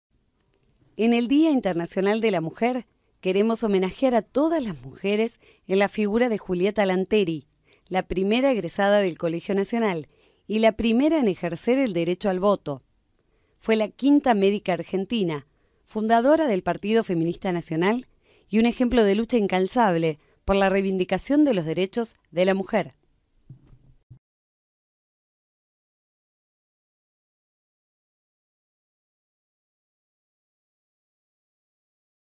Julieta Lanteri - Egresada Ilustre
En el Día Internacional de la Mujer queremos homenajear a todas las mujeres en la figura de Julieta Lanteri, la primera egresada del Colegio Nacional "Rafael Hernández"  y la primera en ejercer el derecho al voto.

Fue la quinta médica argentina, fundadora del Partido Feminista Nacional y un ejemplo de lucha incansable por la reivindicación de los derechos de la mujer.